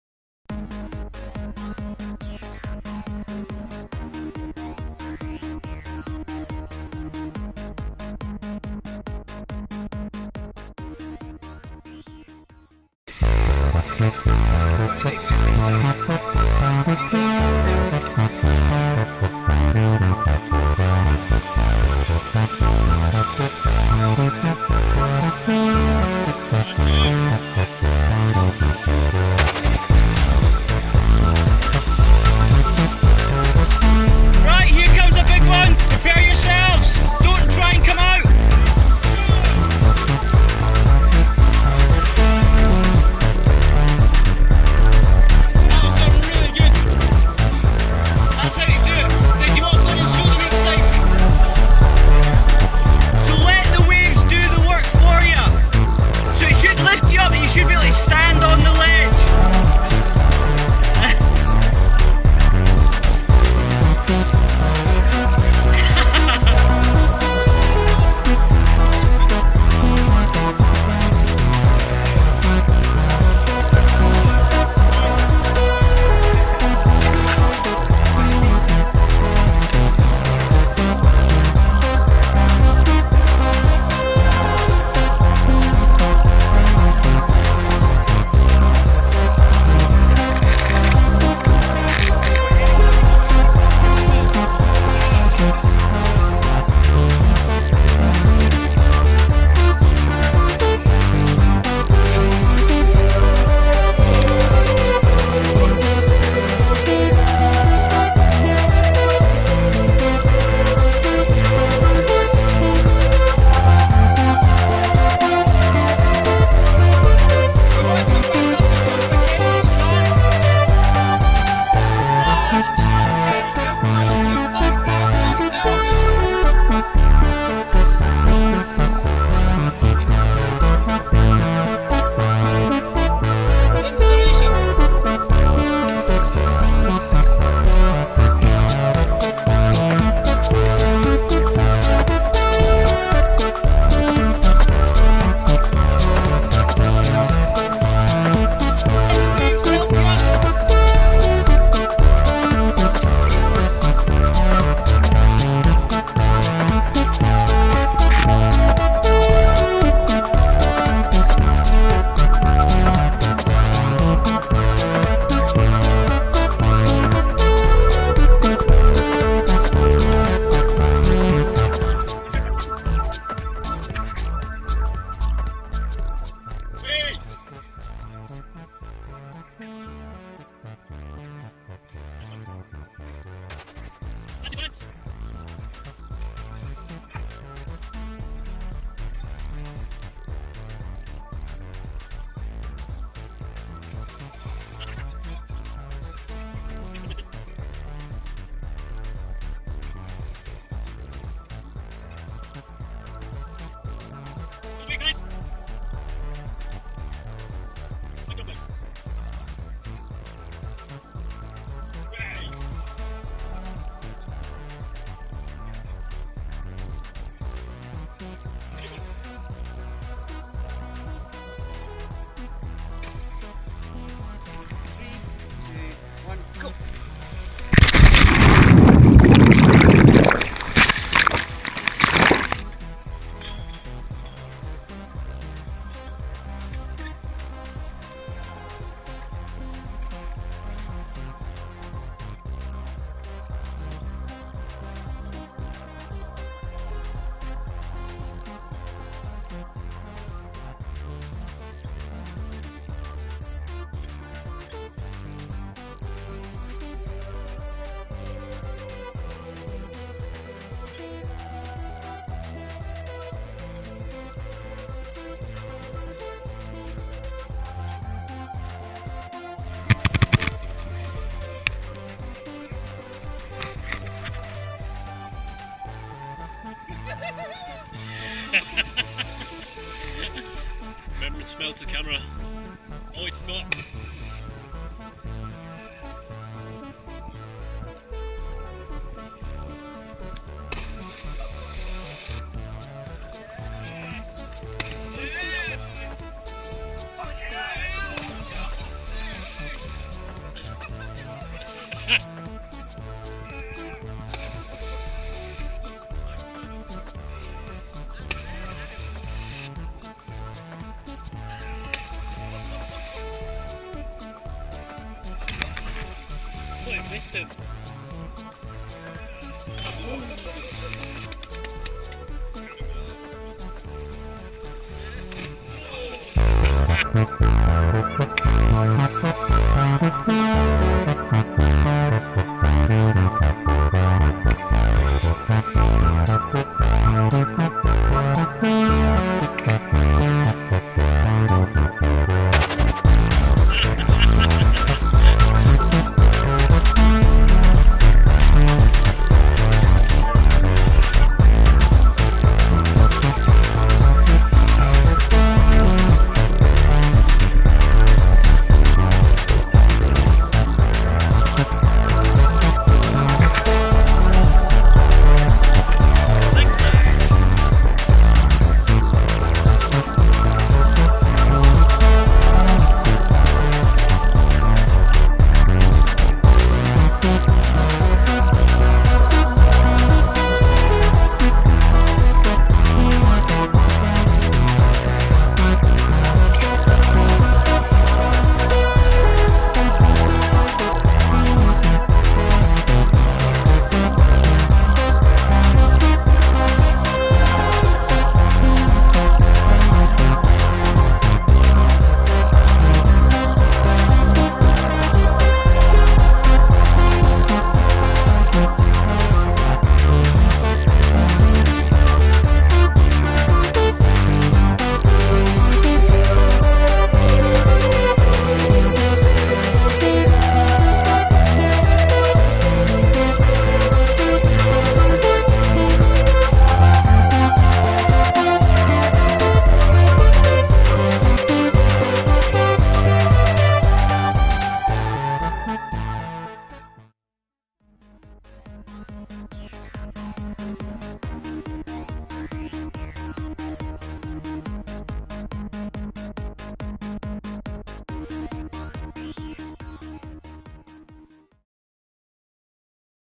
Coasteering Elie Chain Walk
Keen as mustered adventurers take on the Elie Chain Walk and its rugged coastline.
Your never to young or old to have fun.